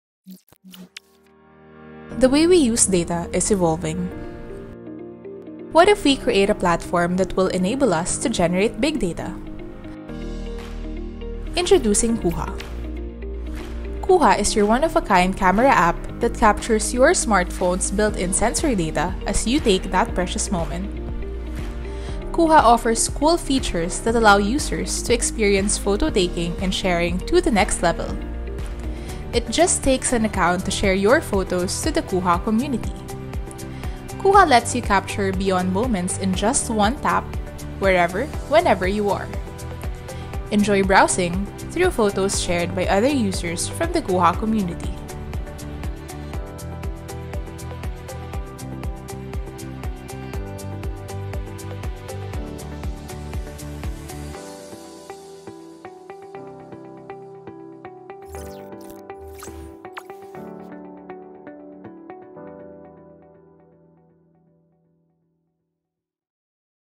Kooha: A Social Sensing Network Application
Ready to capture something new? Meet Kooha.

Kooha is a photo-sharing app developed by the DOST-ASTI which  enables real-time participatory photo and sensor data collection using smartphones and mobile devices. Now ready for download on the Google Playstore, this app can take photos and draw out insights that can help research initiatives in the country.